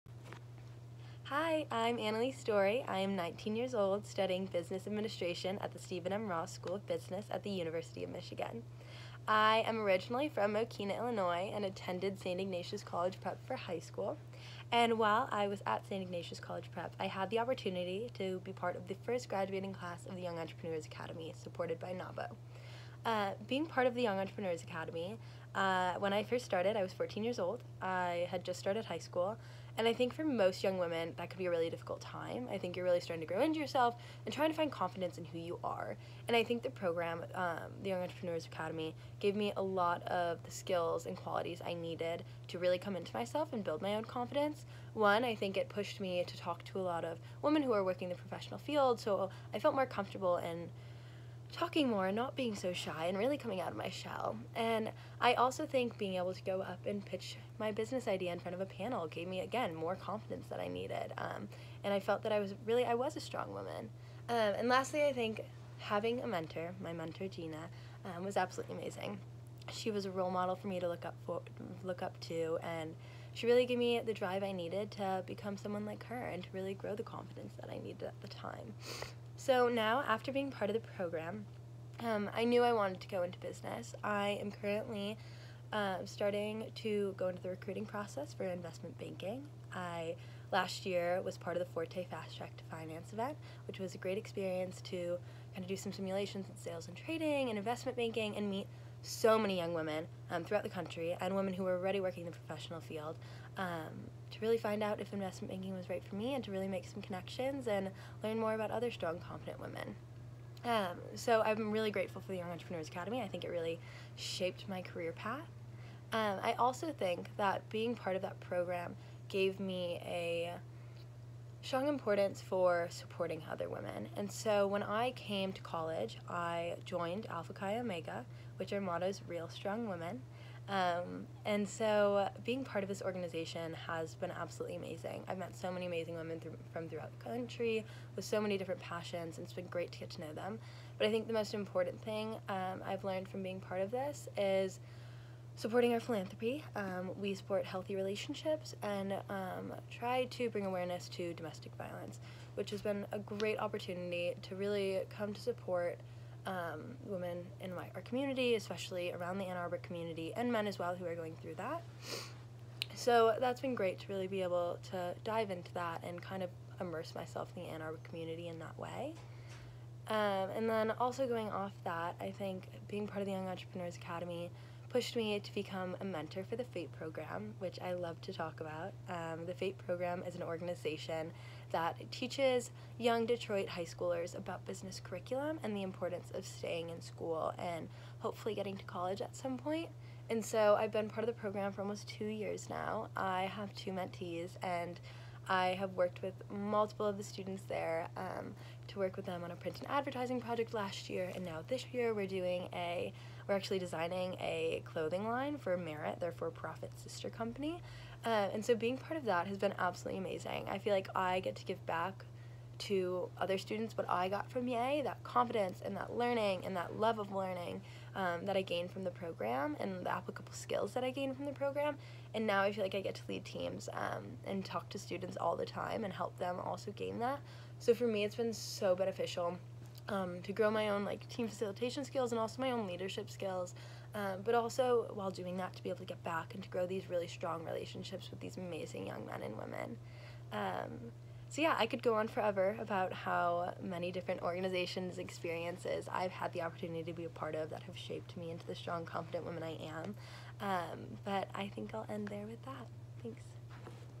YEA! Chicago Alum Analise on How The Program Shaped Her Career Path
Analise Doorhy was part of the first graduating class of YEA! Chicago. Her time in the program and the influence of her mentor helped her, in her own words, "become a strong woman."